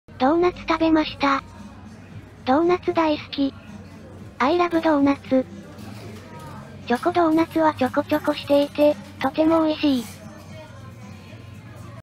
ドーナツ大好き / This is my favorite donut in the world.
ドーナツ大好きな人が撮った動画です。
This is my favorite donut in the world.